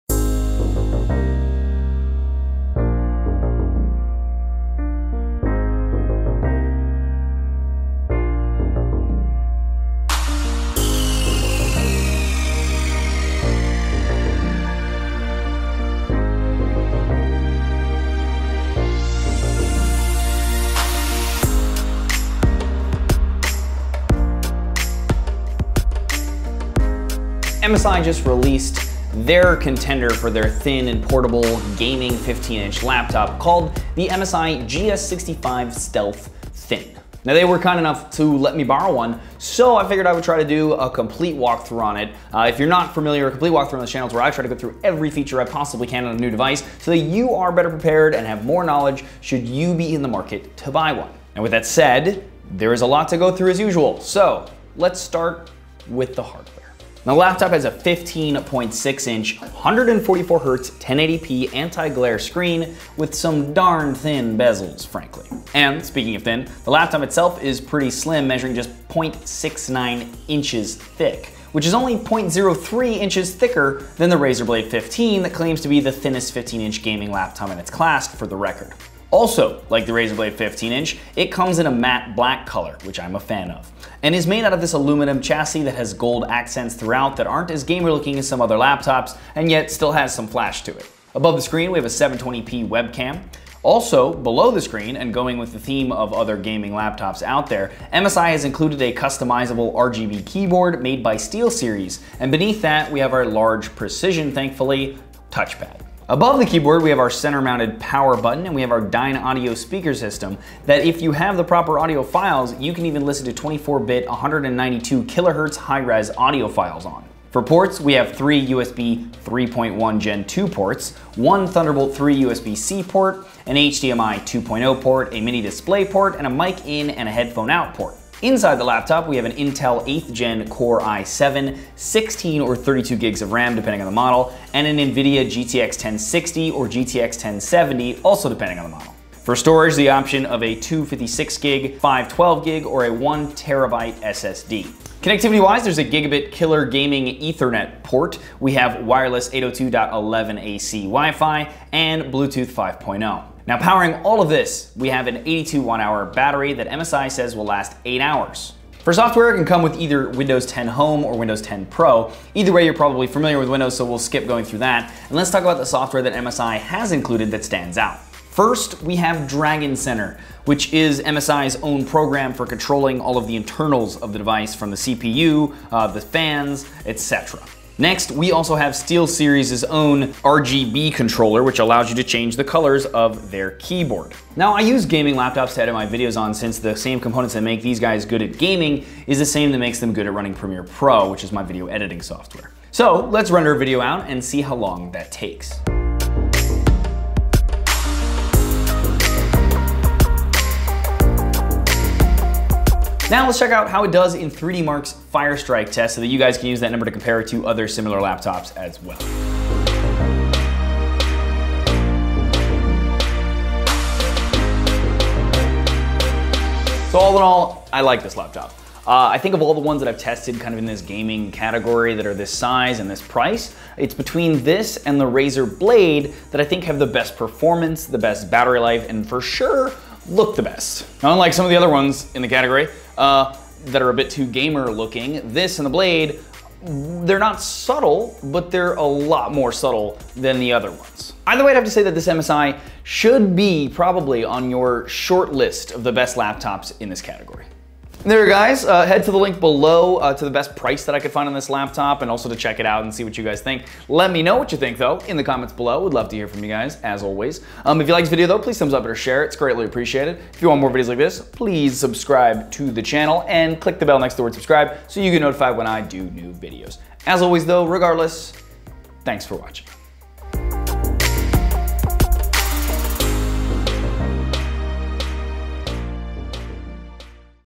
MSI GS65 Stealth THIN Complete Walkthrough: Thin and Powerful Gaming Laptop
MSI released their contender for a thin and portable 15" gaming laptop in the form of the MSI GS65 Stealth Thin.

MSI was kind enough to send me one so I figured I would do a complete walkthrough on it for you guys. If you aren't familiar, a complete walkthrough on my channel is where I go through every feature on a device so you guys are better informed should you be interested in buying one.

HERE'S THE BEST PRICE I COULD FIND ON THE MSI GS65 STEALTH THIN: